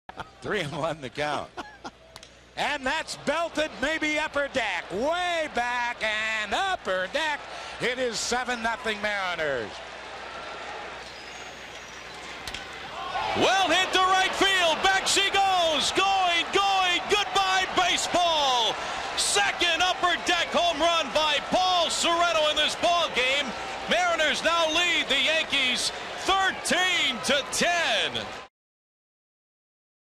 SEA@NYY: Sorrento hits two homers to the upper deck
8/18/96: Mariners first baseman Paul Sorrento hits two home runs to the upper deck in right field at Yankee Stadium